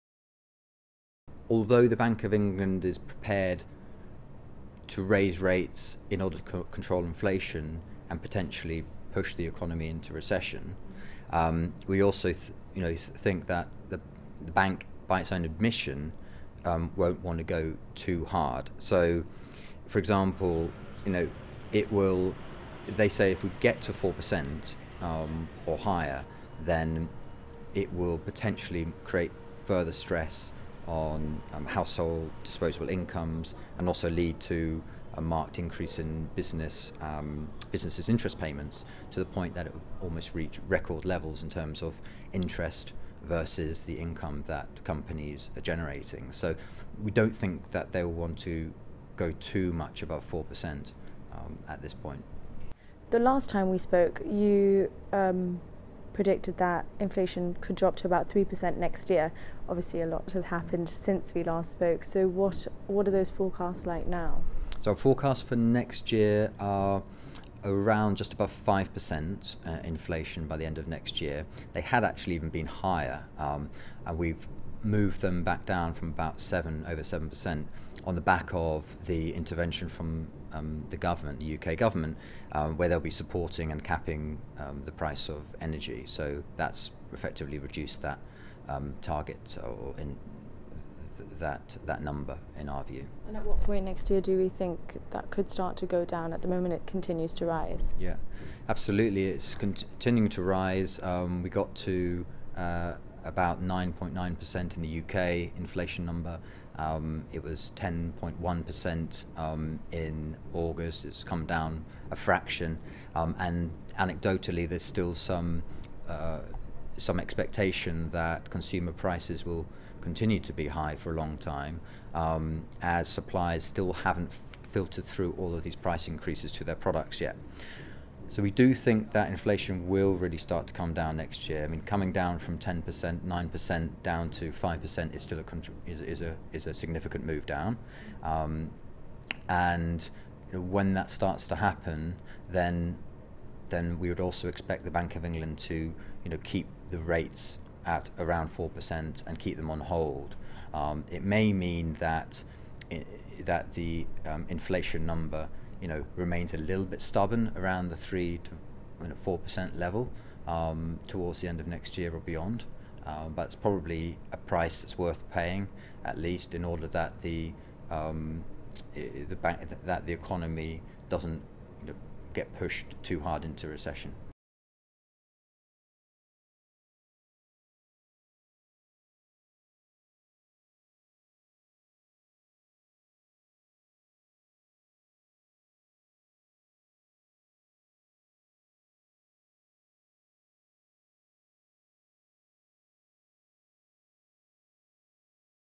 Interest rates will reach about 4% next year & will stop there, according to J Safra Sarasin
Interest rates will reach about 4% next year and will stop there, according to J Safra Sarasin's Tim Rickson. Speaking to GBC on the bank's cost of living forecasts, Mr Rickson anticipated interest rates are expected to reach about 3% by November. These are currently at 2.25%.

Naturally, this will have an impact on the housing market, which is expected to see a drop in demand as a result.